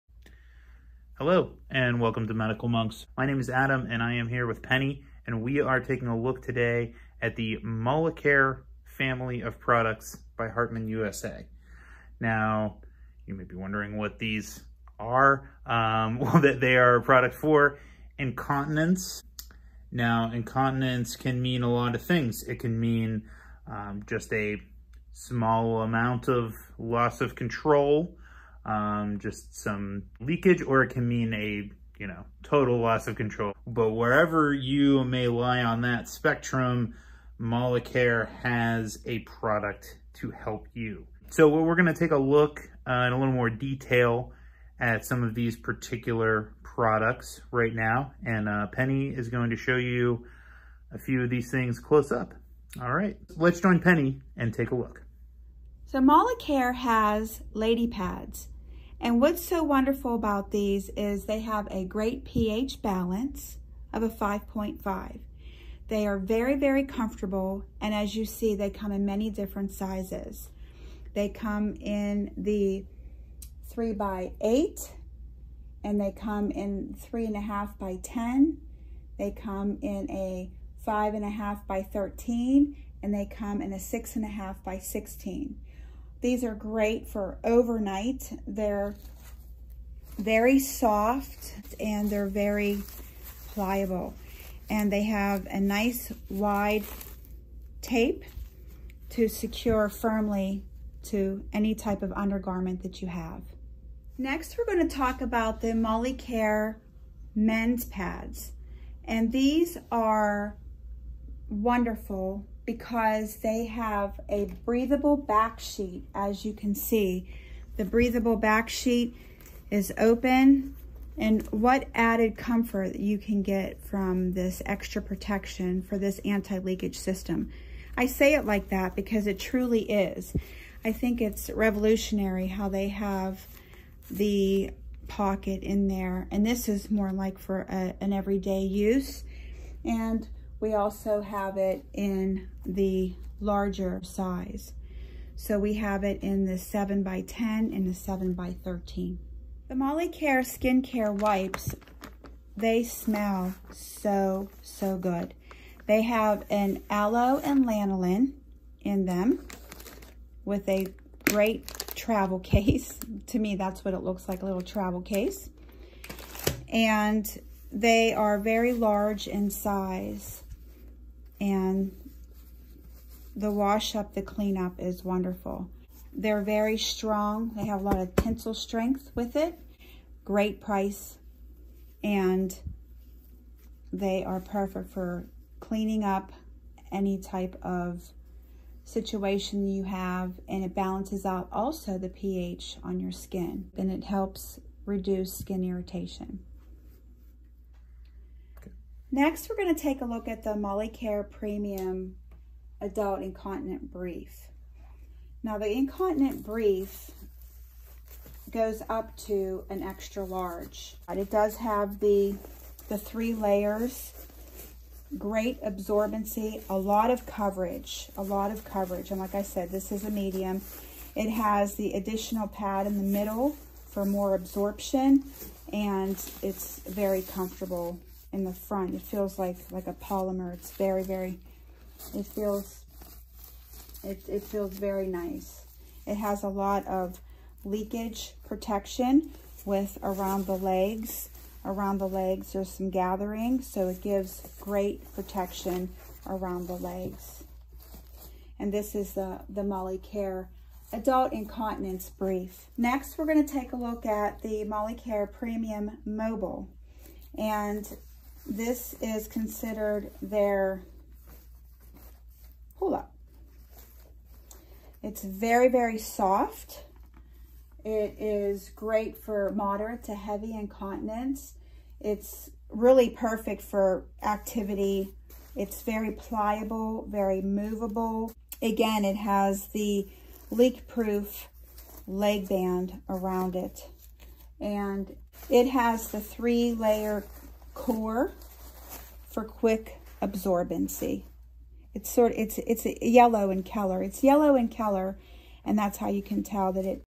Product Line Overview: MoliCare Premium Incontinence by HARTMANN USA | Medical Monks
Join Penny and Adam as they take a look at the MoliCare line of incontinence products from Hartmann USA: “Protecting skin health with a wide range of solutions”

MoliCare® Premium Mobile with ACTIVE SKIN PROTECTION is designed for active adults living with moderate to heavy urinary incontinence. Utilizing innovative Curly Fiber Technology and a three-part highly absorbent core, it helps the skin resist damage by maintaining a skin-neutral pH, and by keeping it dry through superior absorbency A cuff system prevents leakages, the high concentration of superabsorbent polymer neutralizes odors, and the product adapts to the shape and the movements of the body.

MoliCare® Bladder Control Pads with Curly Fiber help manage light to moderate incontinence with ACTIVE SKIN PROTECTION. Skin friendly pH 5.5 ensures maximum skin compatibility. The 3-layer absorbent core quickly acquires urine, keeps the skin dry, and provides superior leakage protection with high pad integrity. Adhesive strip allows placement in personal undergarment.